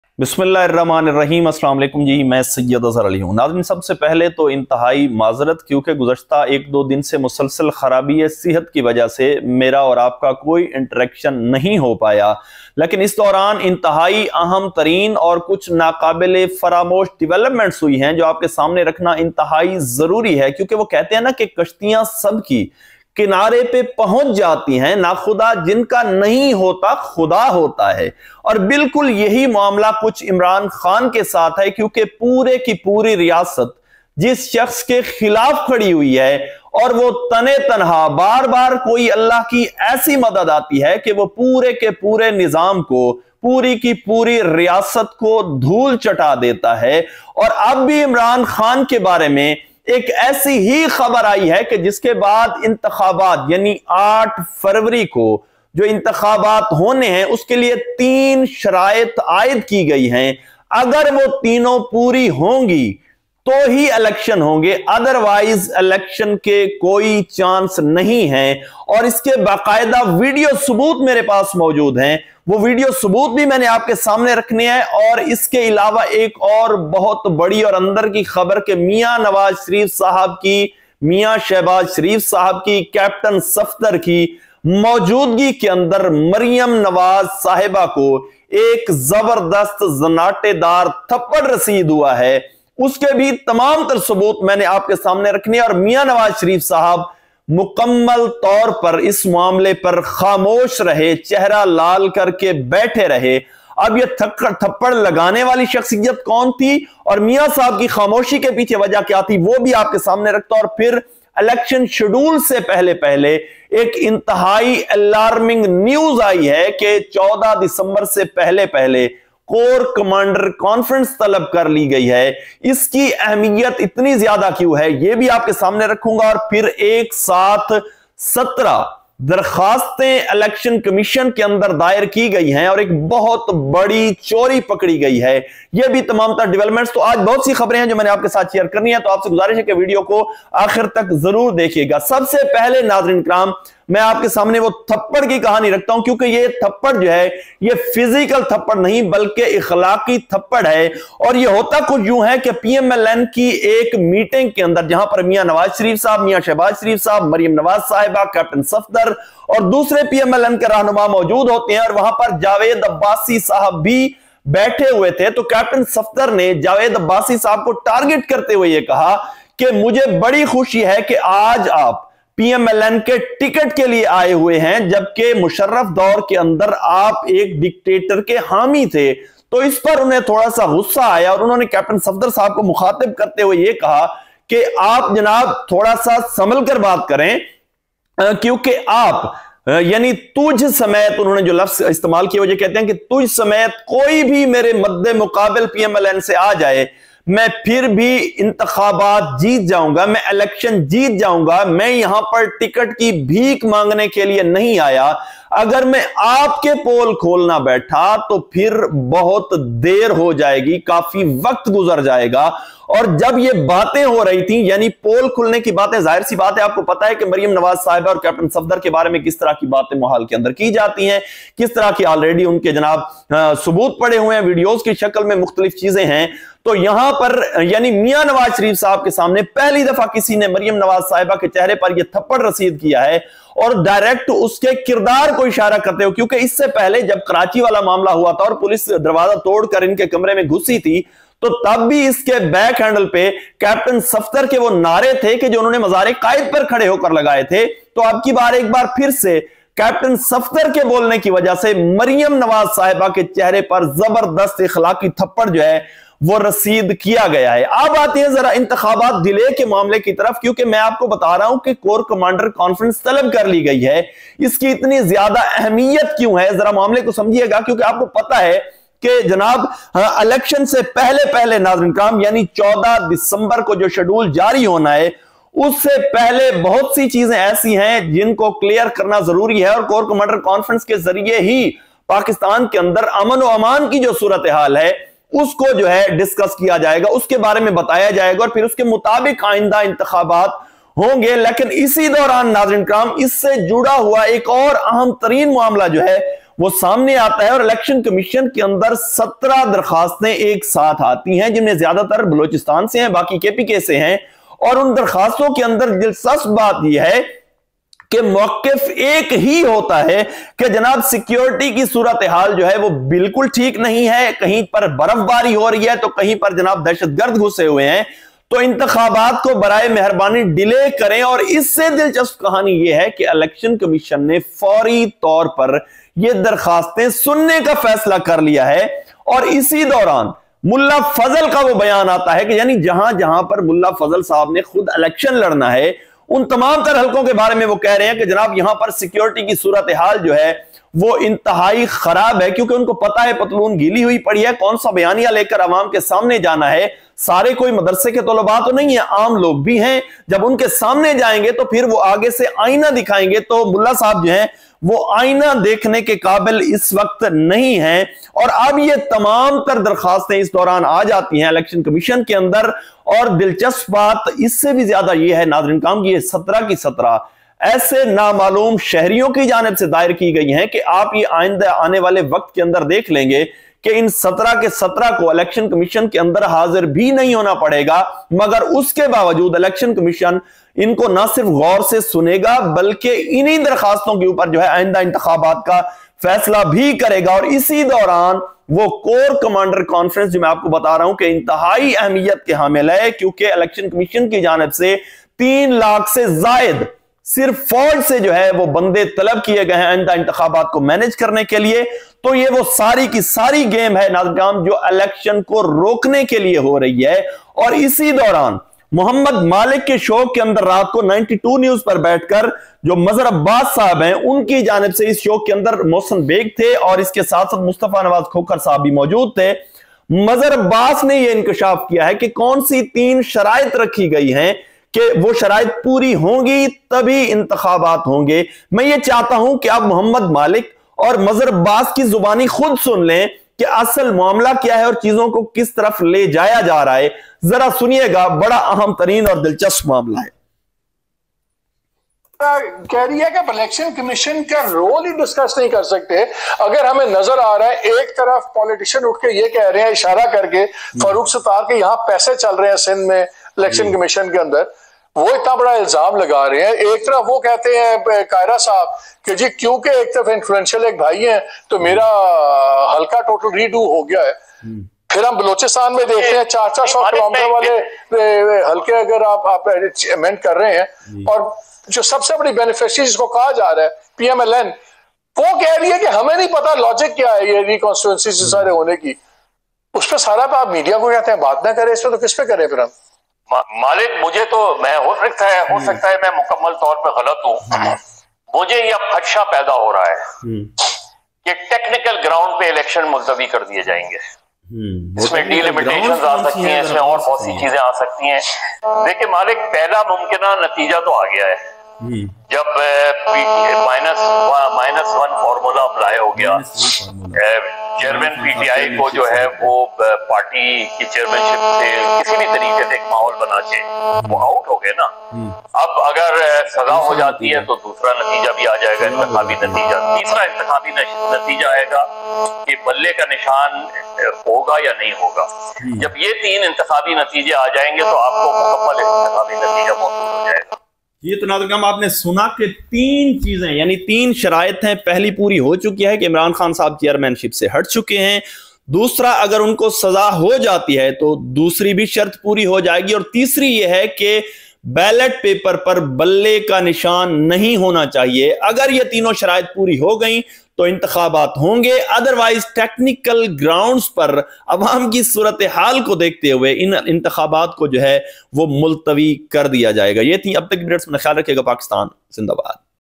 Maryam Nawaz Slapped in the presence of Nawaz Sharif | Elections 2024 | 17 Applications in EC
انتخابات ایک ہی صورت میں!یہ تین کام کریں!!ویڈیو ثبوت
نوازکی موجودگی میں مریم کو تھپڑ!مگرخاموشی
الیکشن شیڈیول سےپہلےپہلےہنگامی کورکمانڈرکانفرنس